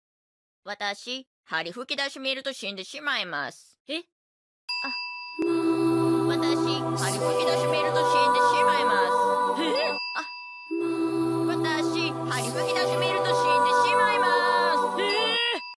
When Frosty sees a jagged Manga balloon...
...he dies.
A lot.

Started watching Pop Team Epic yesterday...it does inspire me to animate, but nothing good.

Have Fun!